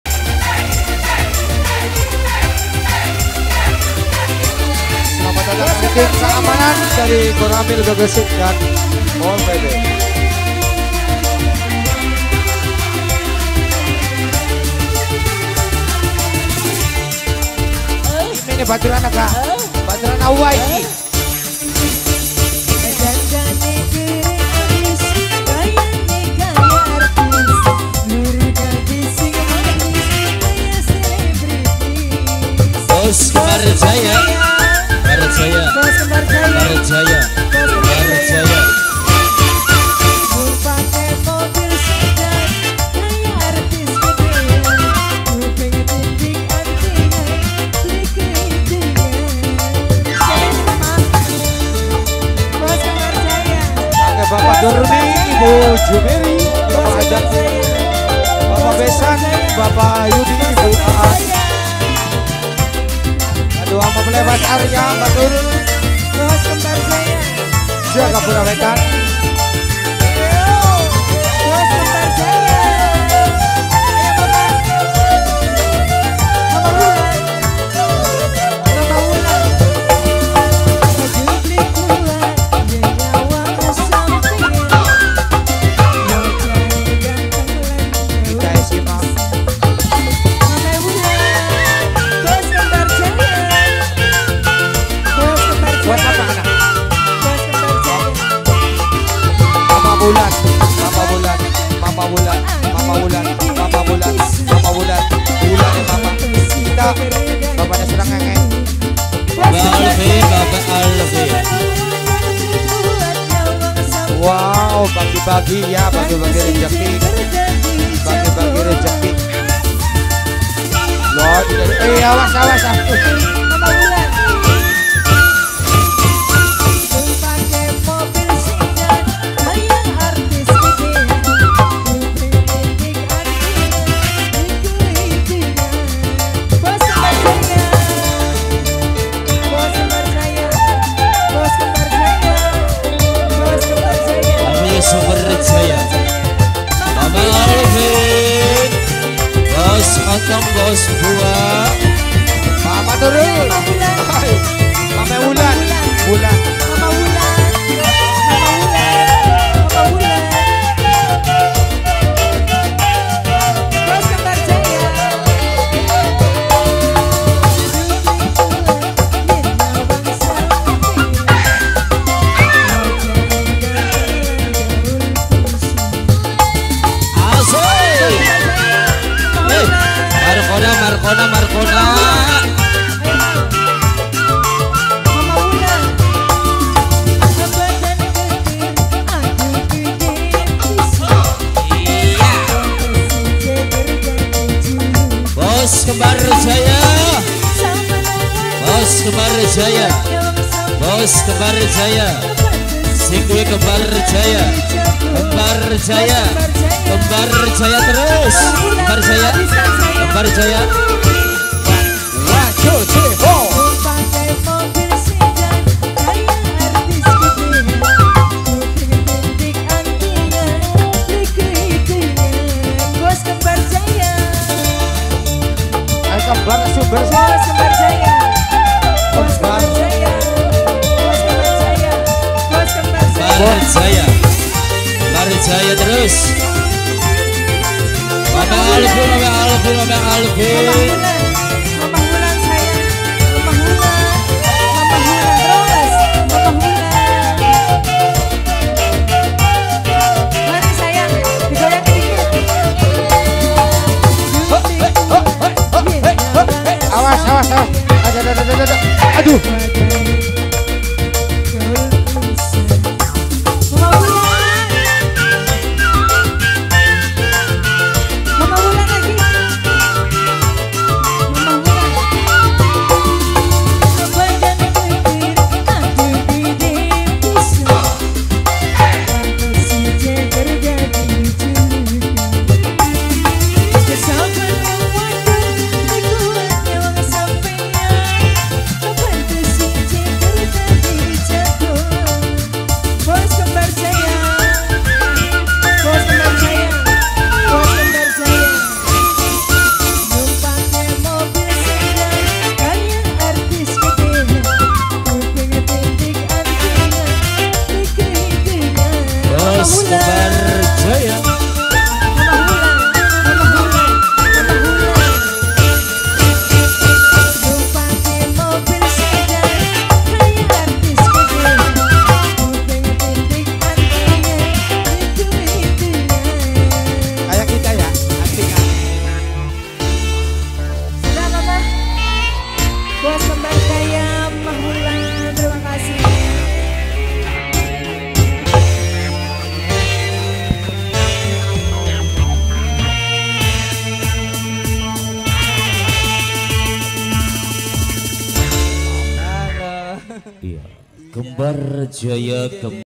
JEGER // NEW JG MUDA ENTERTAINMEN //JAGAPURA WETAN - GEGESIK - CIREBON
Documentasi Pernikahan " Arya & Nurul " Pemangku Hajat Bpk.Durmi & Ibu Jumeri
Gang Tumpal RT.01 RW.04 Ds.Jagapura Wetan
Kec.Gegesik - Kab.Cirebon Pelaksanaan Rabu ,25 Oktober 2023